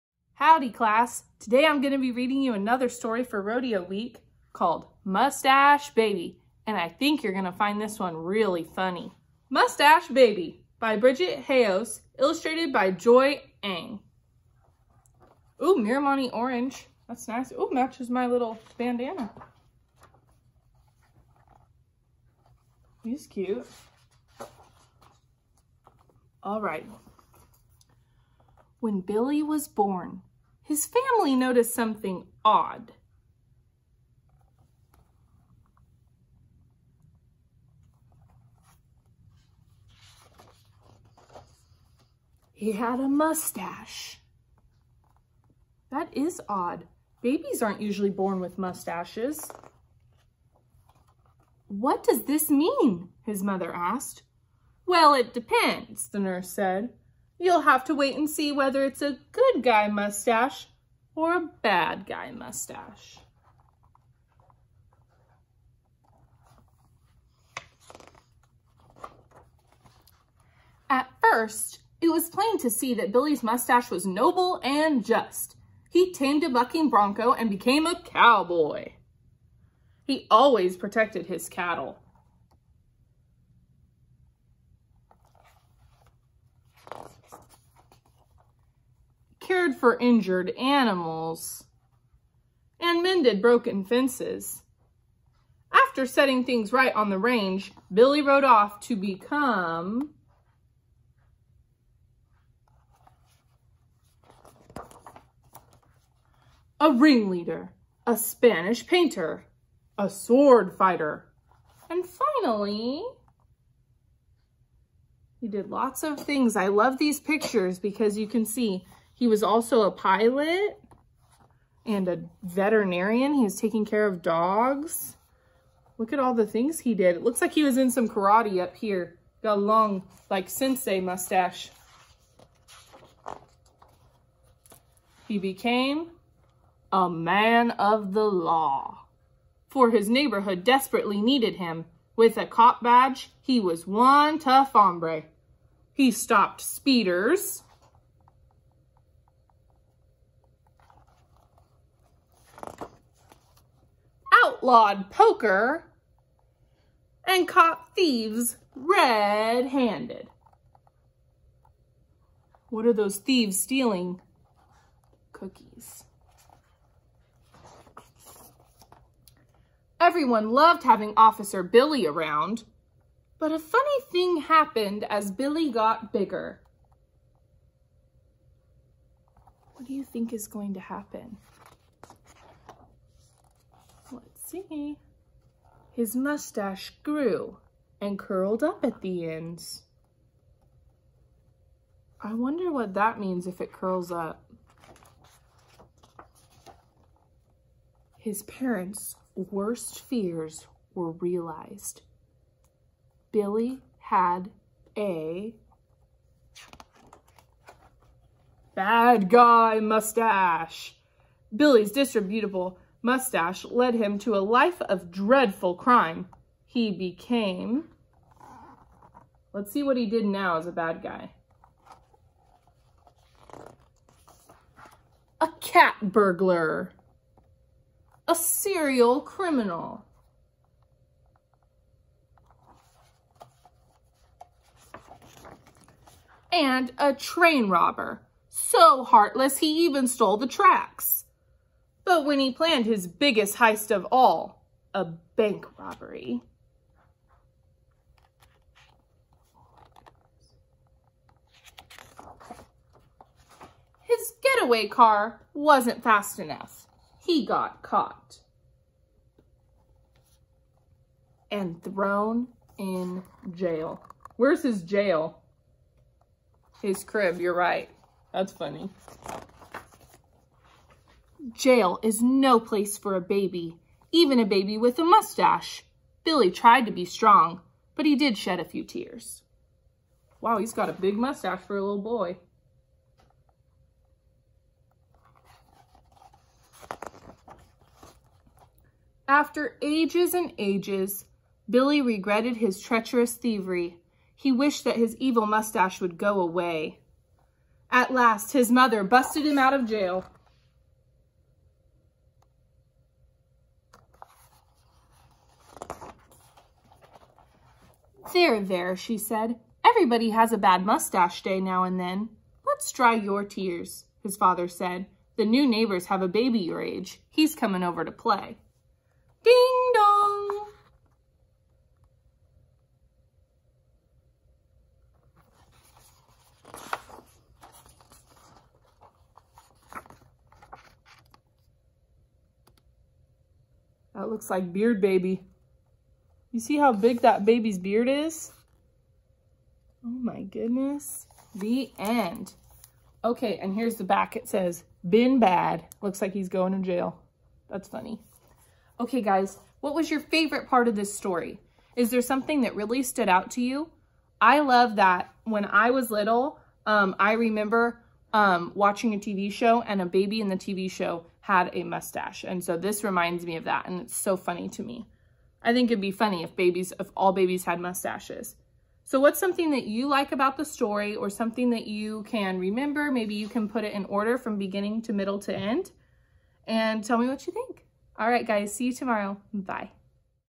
Crafty In Kindergarten | Miss Perry reads "Mustache Baby"
QuestionOfTheDay Do you know someone with a mustache? My dad AND my grandpa have a mustache!

Howdy Y'all! Today I am reading you a funny story about this baby that was born with a mustache! After this video, check out the "Wanted Poster" video! I would love for you to complete a wanted poster with me! What are you going to be caught red handed doing (positive)?